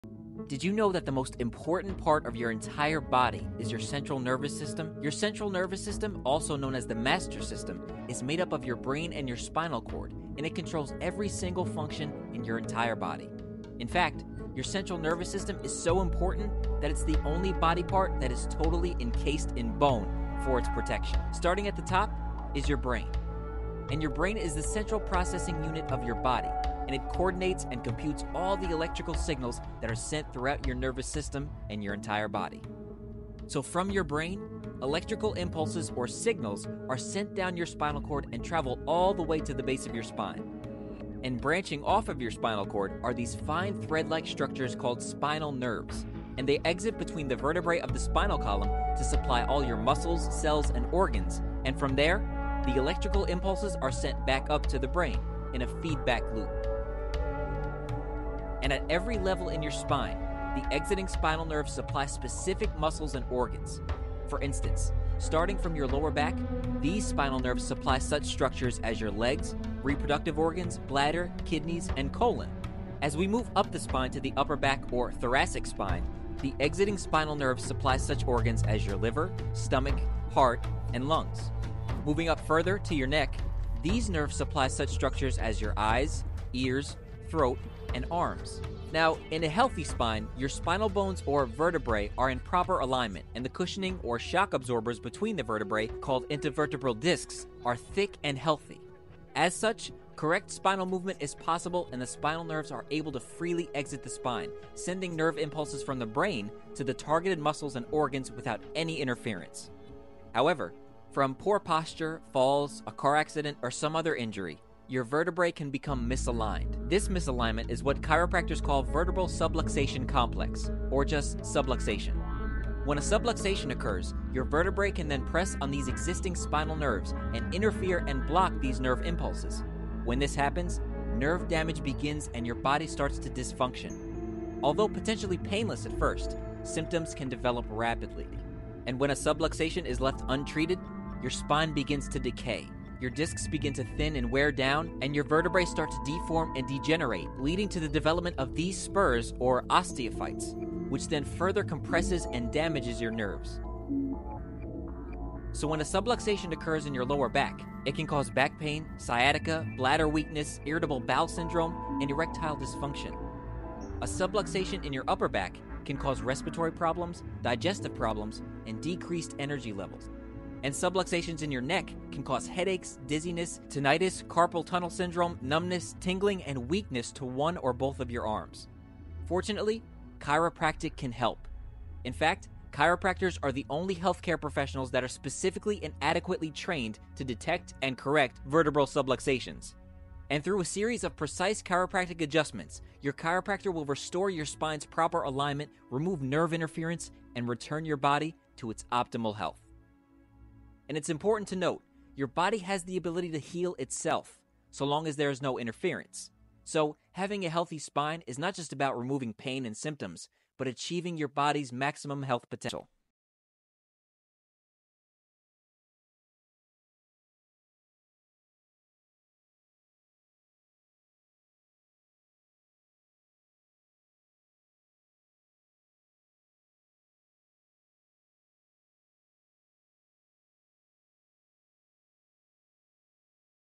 What is the Central Nervous System ? Anatomy and Function | Animated Video
🧠 Exploring the Central Nervous System: Anatomy and Function 🧠

Welcome to our latest video where we dive into the fascinating world of the central nervous system (CNS)! In this comprehensive animation, we’ll explore:

🔬 Structure of the CNS- Learn about the brain and spinal cord, their roles, and how they work together to control the body.

🧩 Key Functions - Discover how the CNS processes information, coordinates movement, and regulates bodily functions.

🧠 Neural Pathways- Understand the complex network of neurons and how they communicate to keep us functioning smoothly.

🎨 Interactive Animations - Visualize the intricate details of the CNS through engaging and easy-to-understand animations.

Whether you’re a student, medical professional, or just curious about how your body works, this video will provide valuable insights into one of the most critical systems in the human body.